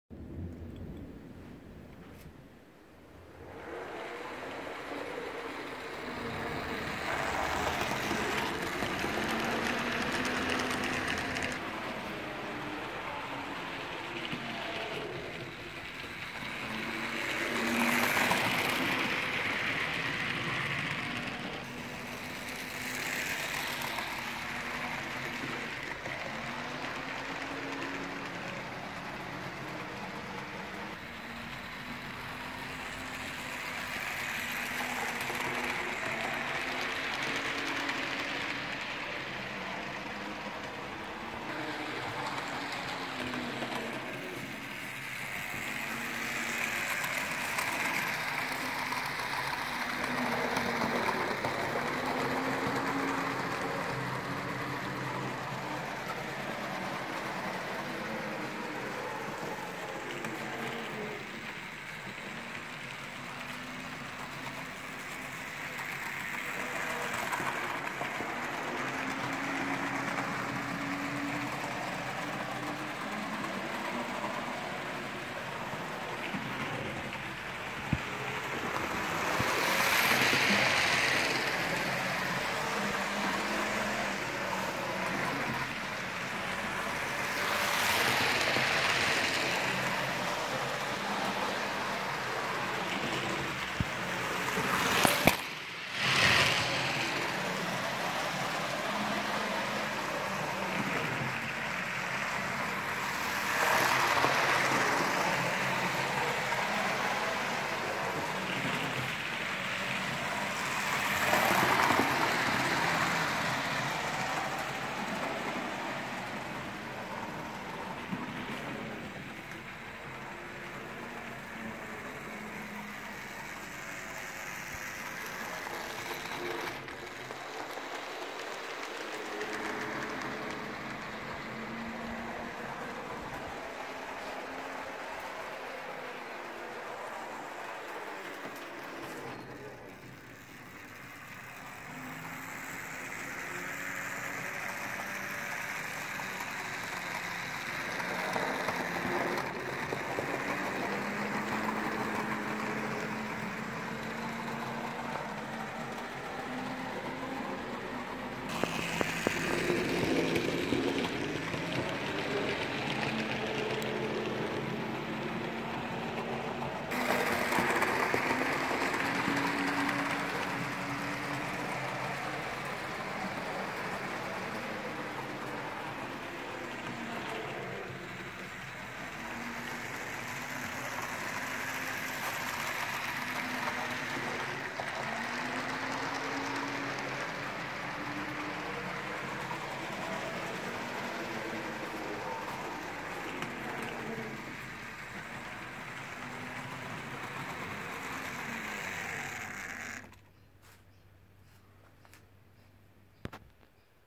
Triang 1968 Flying Scotsman with rake of 4 clerestory coaches.
This FS has hardly been used. In near mint condition with firebox glow.